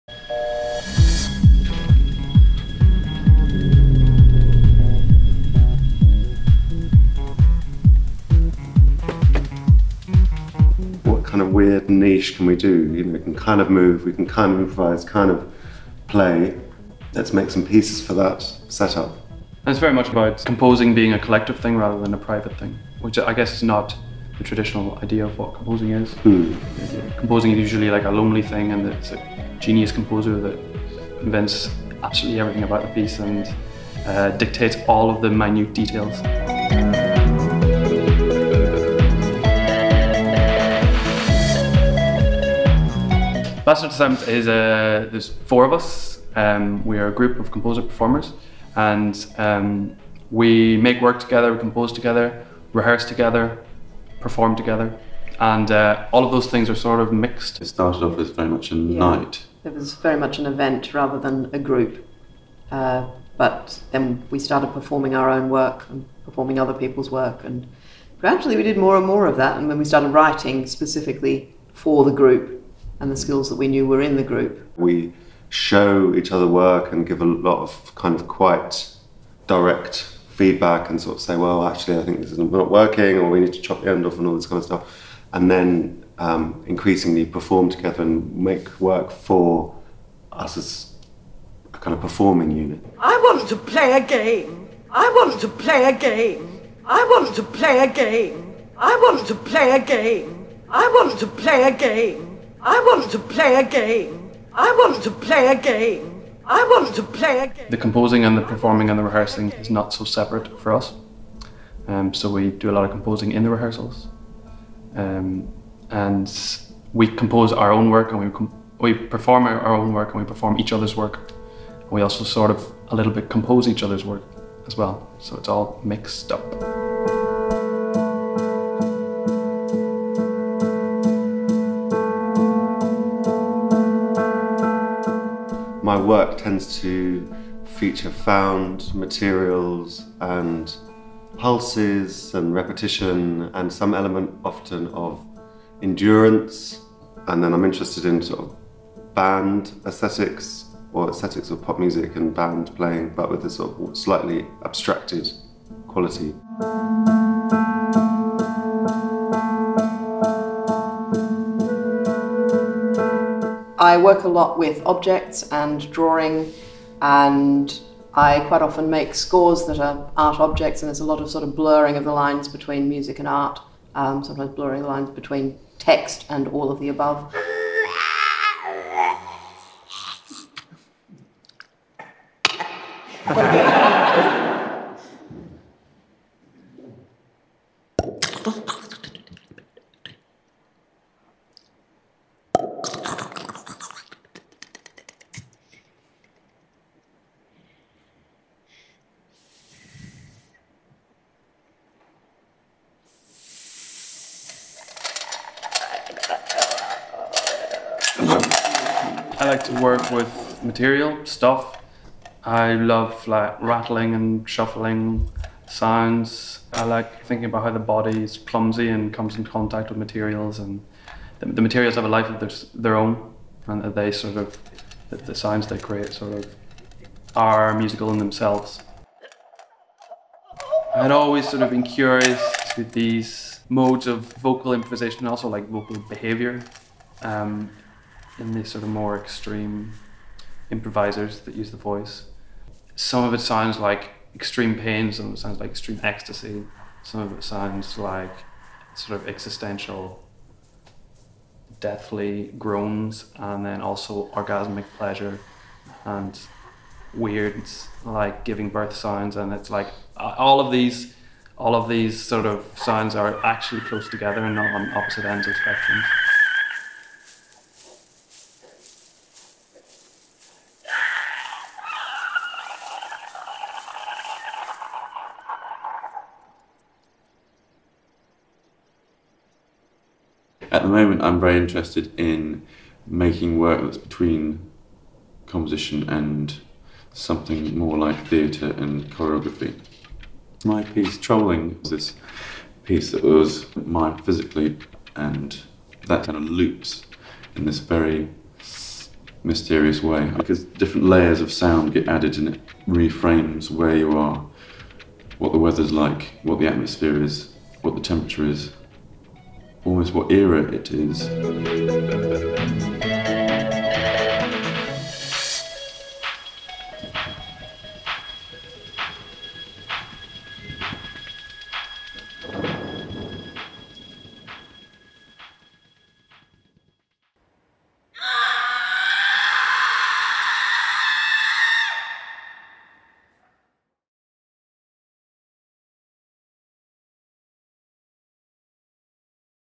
Bastard Assignments • Festival of New 2018 • Open Space • Snape Maltings
Bastard Assignments are Timothy Cape, Edward Henderson, Caitlin Rowley and Josh Spear, a collective of composer-performers who produce events. Since 2016 they have been developing a shared creative practice, collaboratively developing their work as composer-performers.

Bastard Assignments perform at Aldeburgh Festival on Saturday 15 Jun 10:15pm

See more about the first Festival of New at our